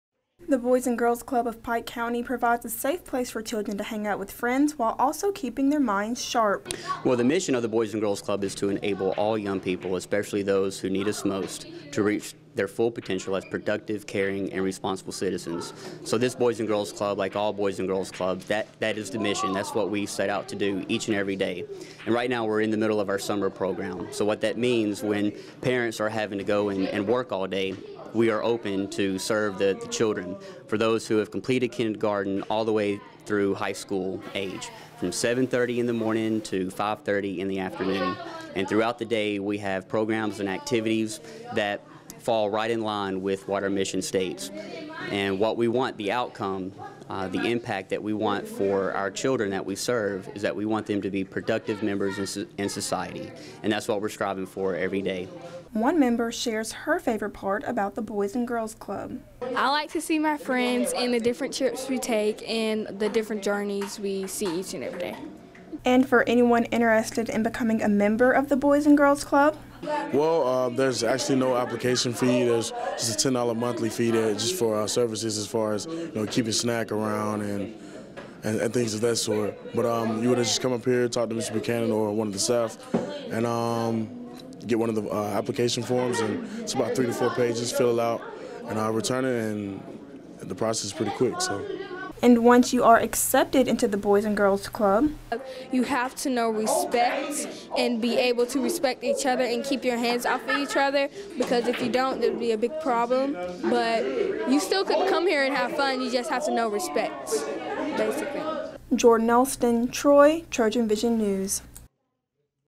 Boys and Girls Club - TROY TrojanVision News
Summer is in full swing, leaving many young Pike County residents searching for some fun activities.  TROY TrojanVision News' Jordyn Elston shows us one place that is offering a lot of summer fun.